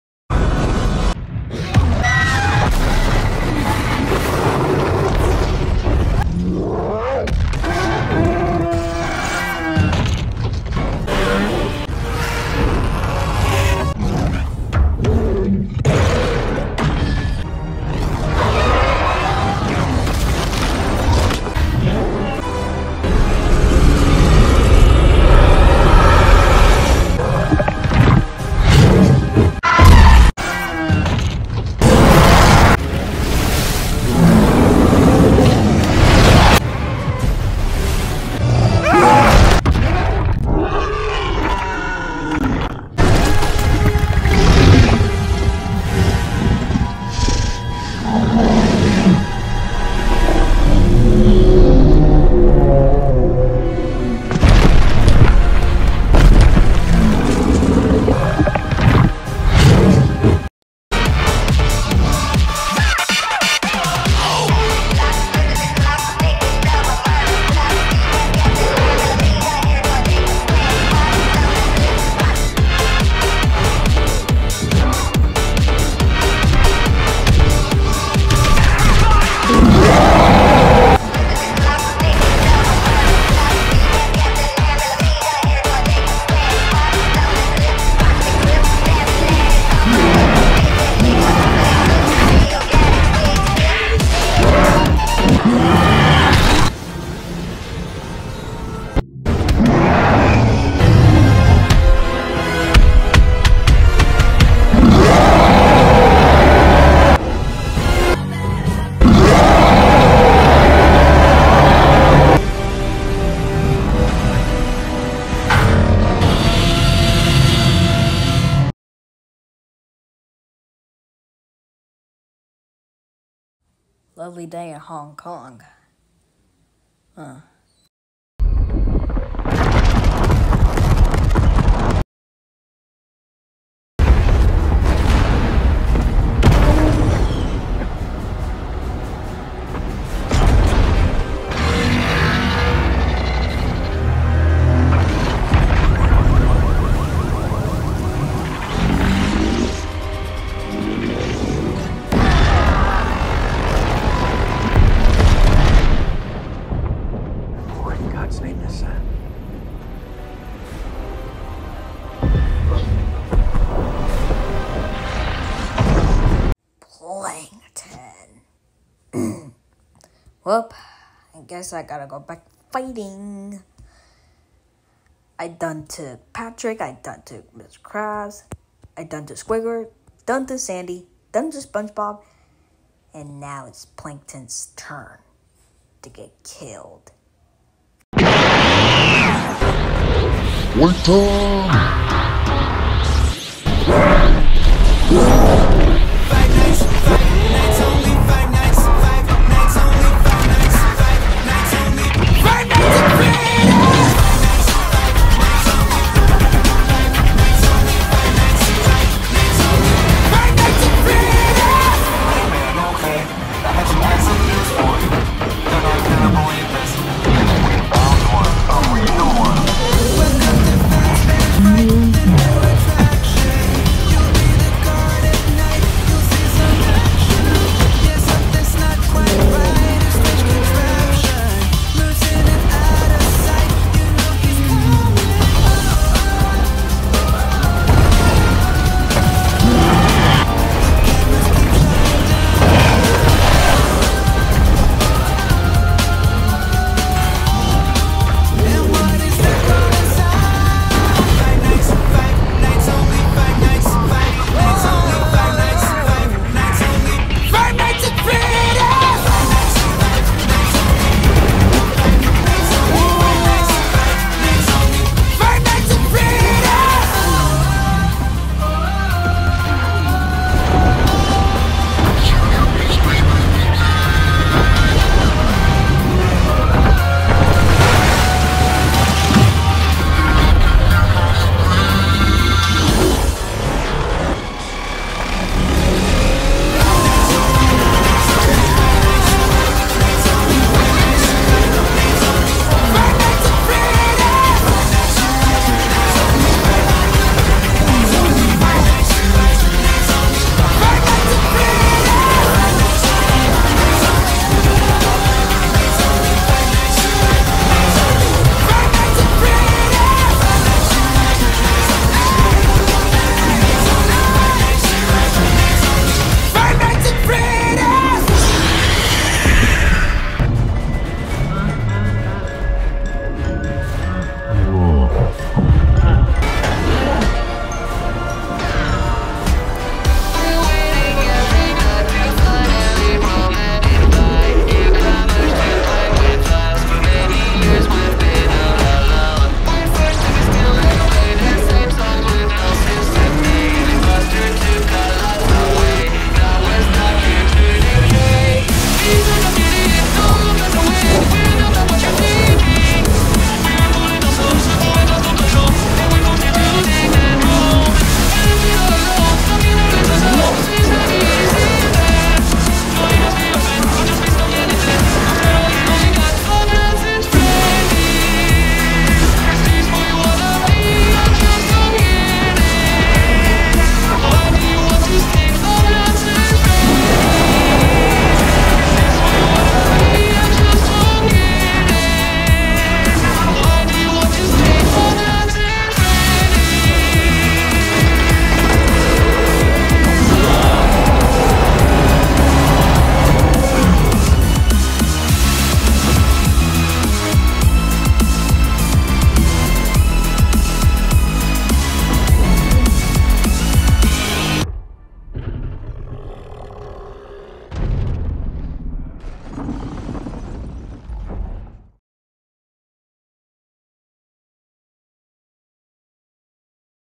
King Gio vs Mecha Planktonzilla
Movies

This is the final SpongeBob Kajiu battle